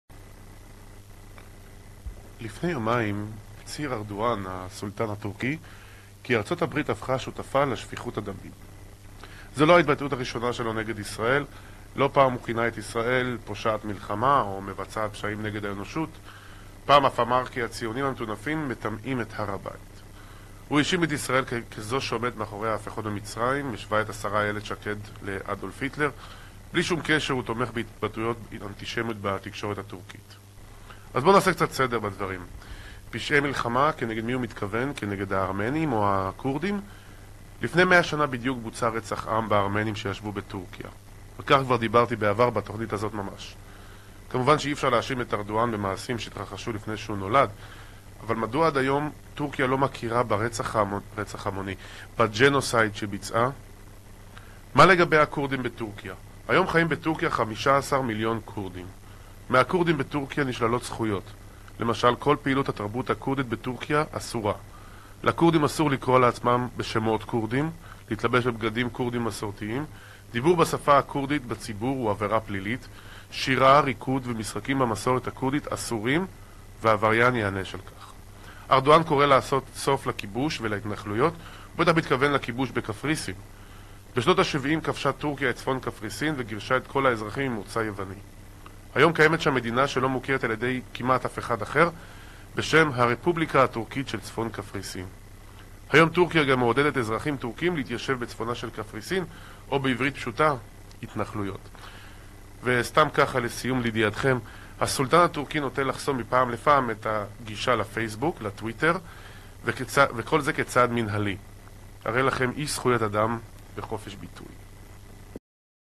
ארדואן - תסתכל על עצמך ועל ההיסטוריה שלך: רצח עם, כיבוש, אפרטהייד והתנחלויות
ארדואן - תסתכל על עצמך ועל ההיסטוריה שלך: רצח עם, כיבוש, אפרטהייד והתנחלויות. איתמר צור במונולוג על הסולטאן הטורקי